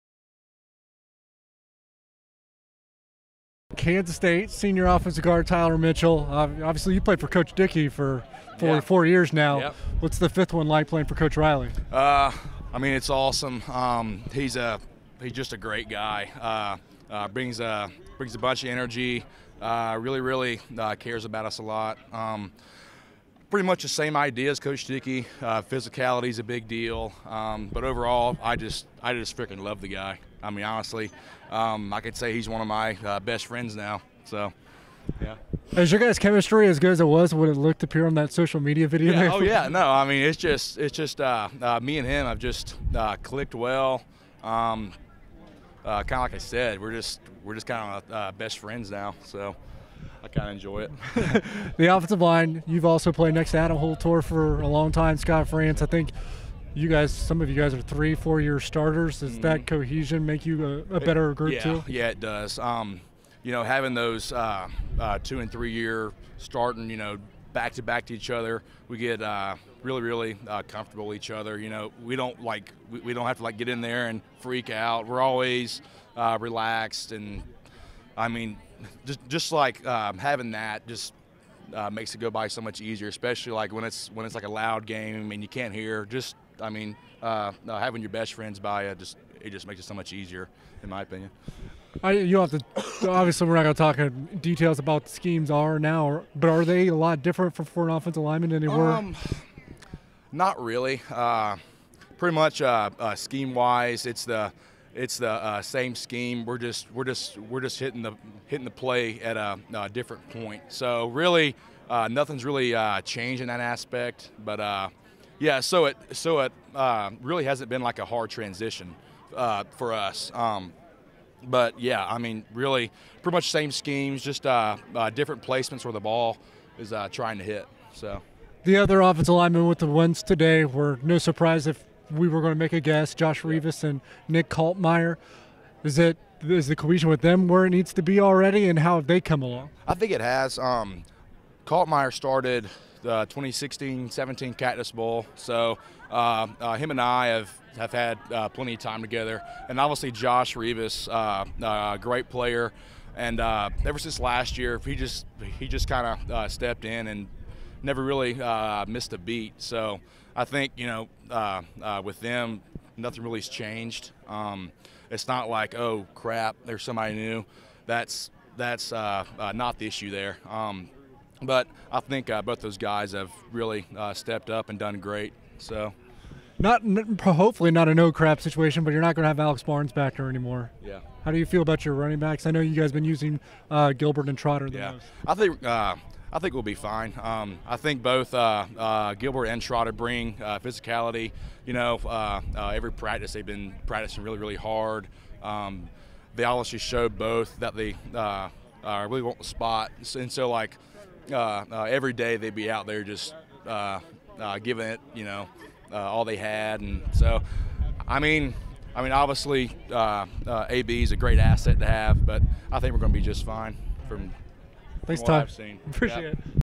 Kansas State Wildcats Football Spring Showcase: Tyler Mitchell - 4/13/19
K-State OL Tyler Mitchell speaks to the media after the Spring Showcase.